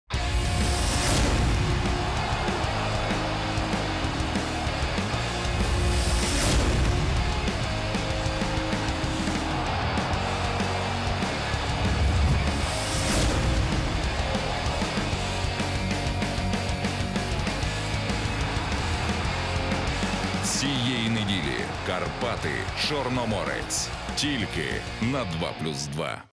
Телеканал 2+2. ЧУ 15 тур. Карпати - Чорноморець
Дивіться пряму трансляцію матчу 15 туру Чемпіонату України з футболу "Карпати" - "Чорноморець" у неділю, 03 листопада, о 19:20 на телеканалі "2+2".